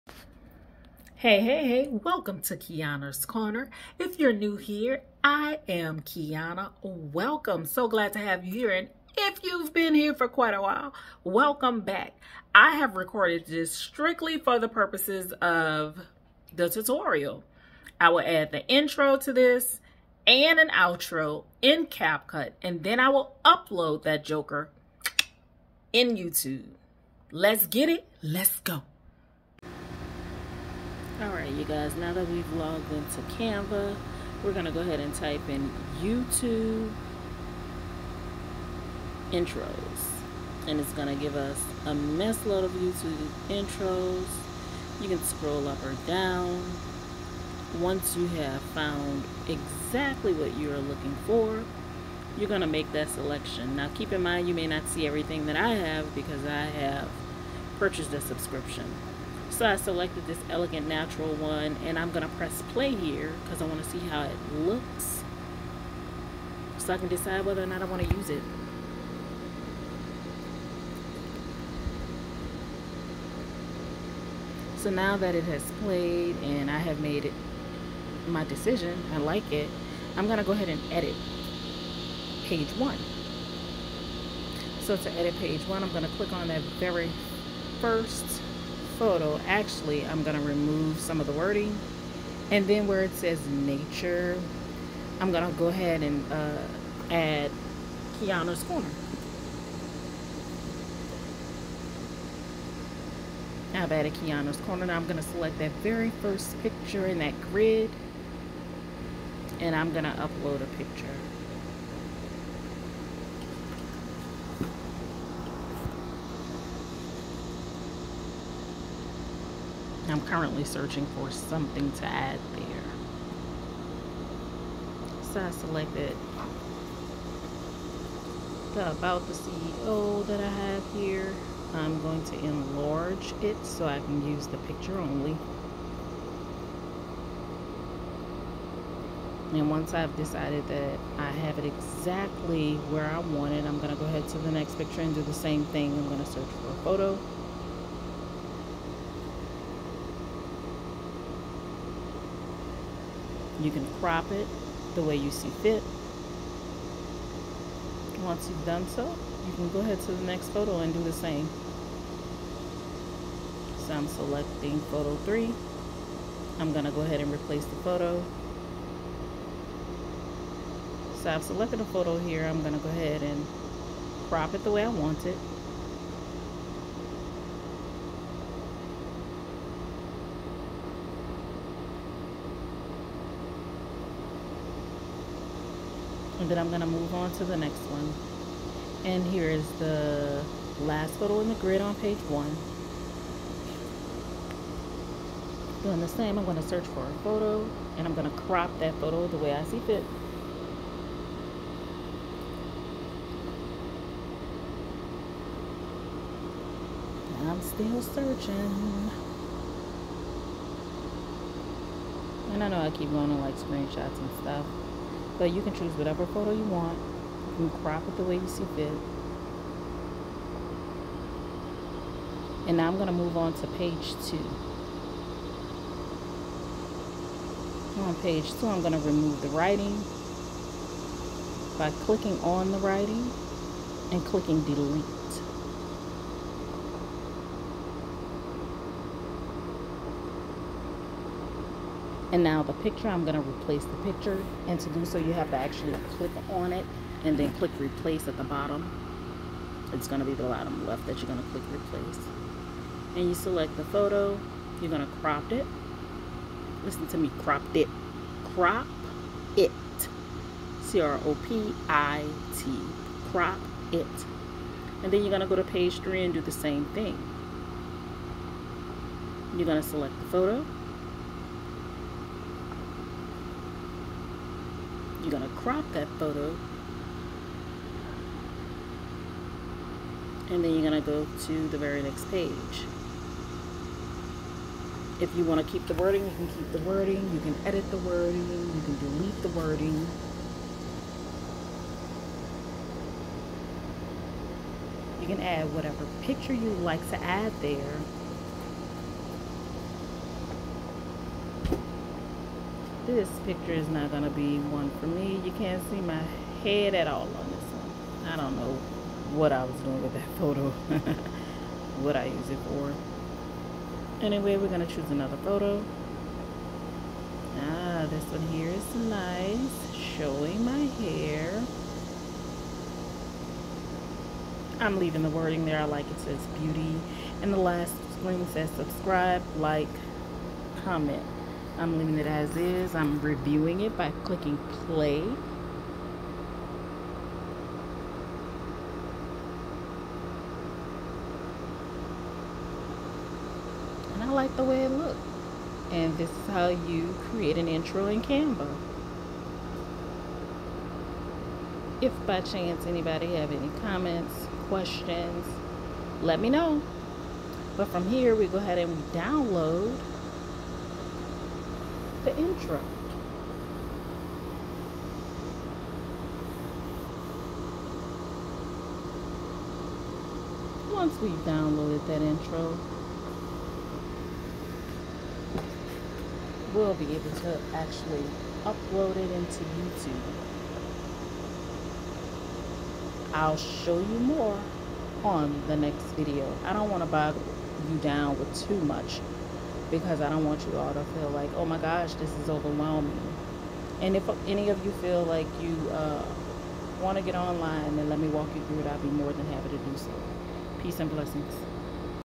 Create a YouTube Intro in Canva | Step-by-Step Tutorial
Want to create a YouTube intro right from your Android device? In this video, I’ll walk you through the entire process using Canva, a powerful and easy-to-use design app. Whether you're a beginner or just looking to make your channel stand out, this tutorial will show you how to design a professional-looking YouTube intro in just a few minutes.